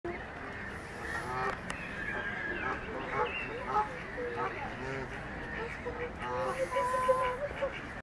Wild Hare among Ducks
Wild Hare eating among wild ducks and ducklings